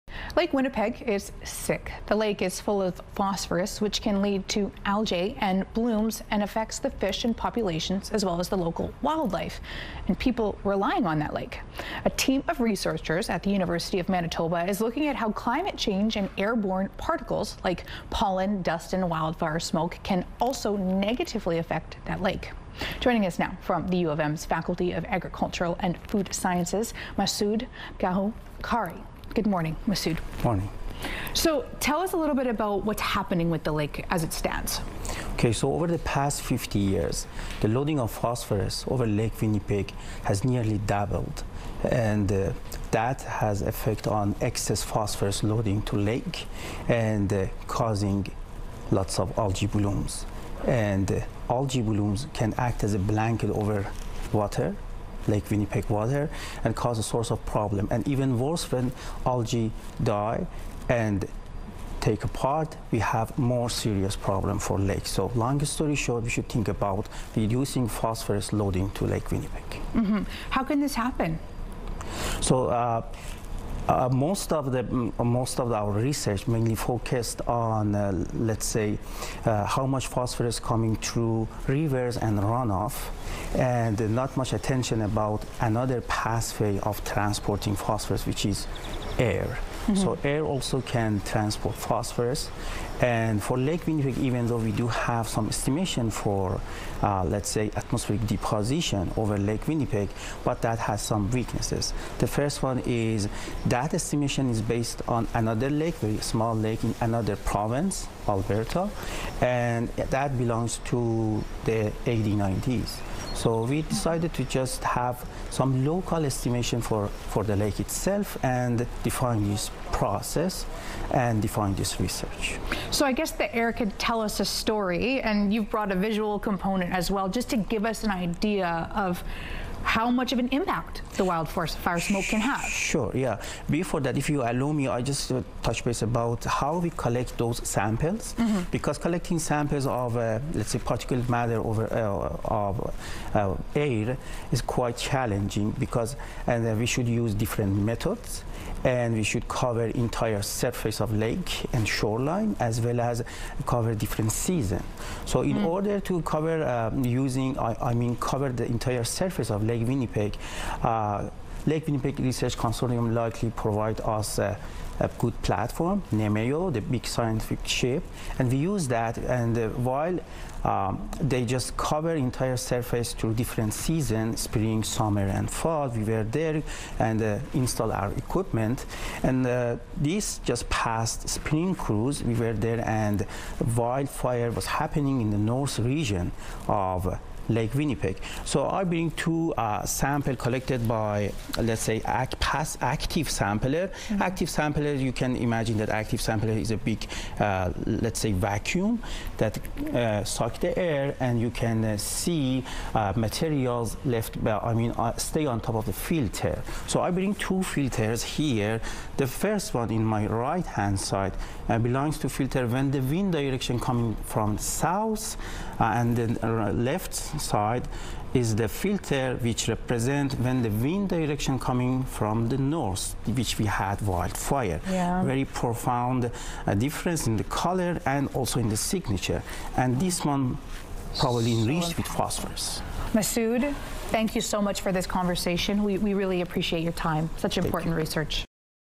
New research targets Lake Winnipeg’s phosphorus pollution
Masoud Goharrokhi from the University of Manitoba discusses a project studying airborne phosphorus to help improve water quality in Lake Winnipeg.